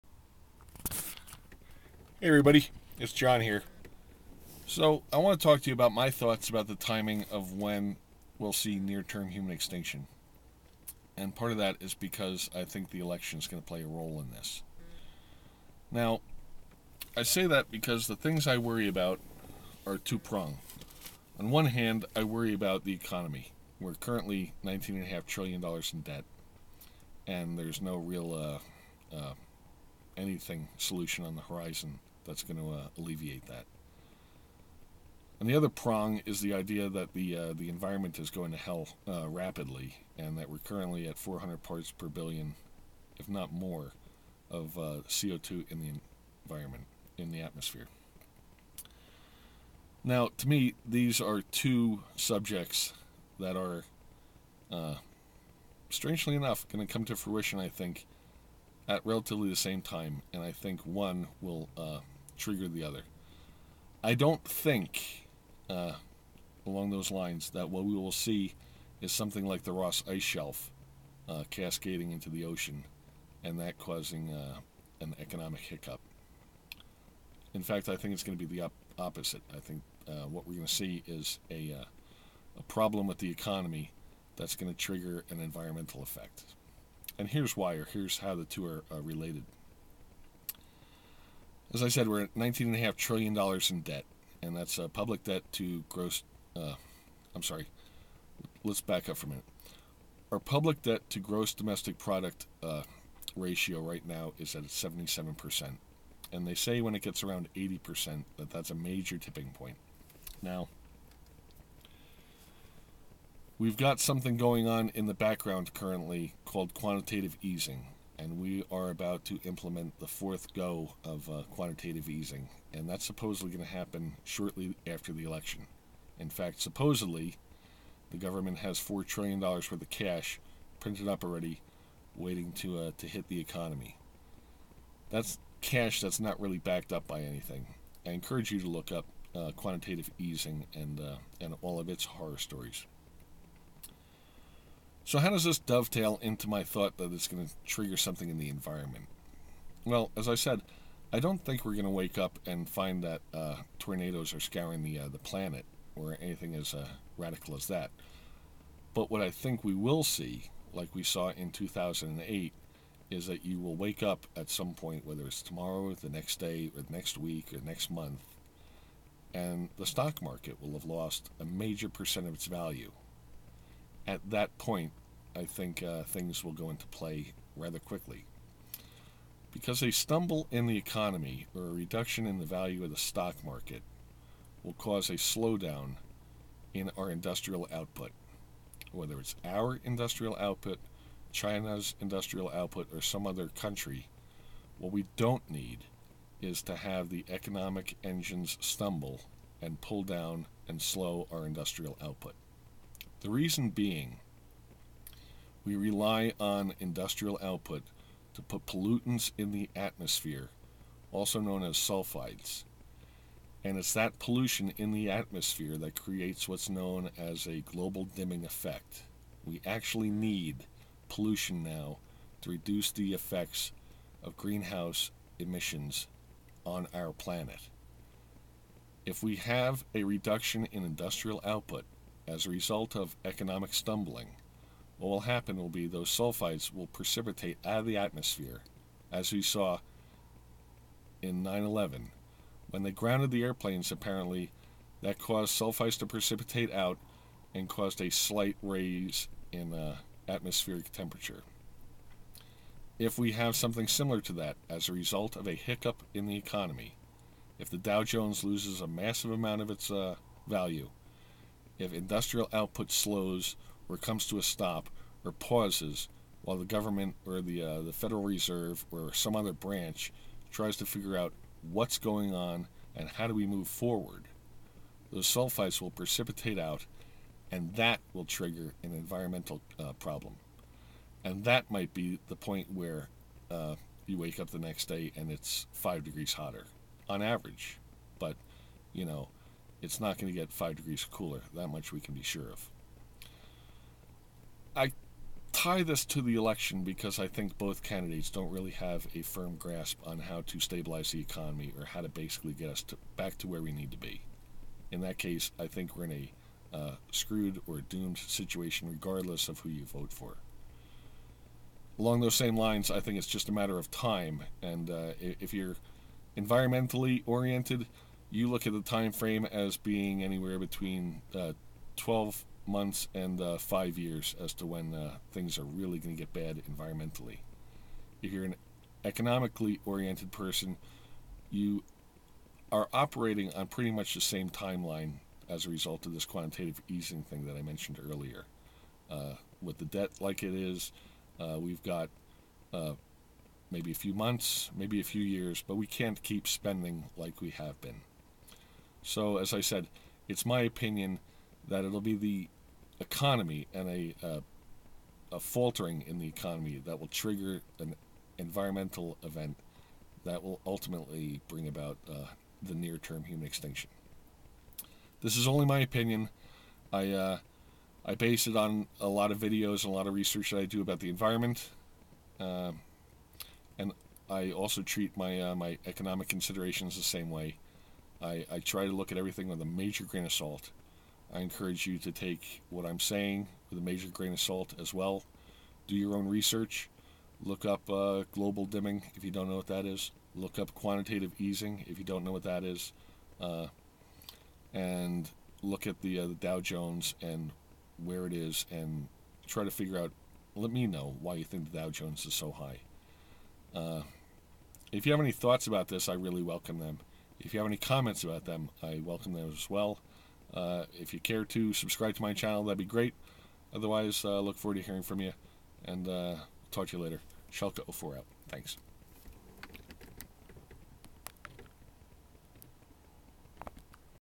NTHE and the Election
My thoughts on how recent election results may help trigger NTHE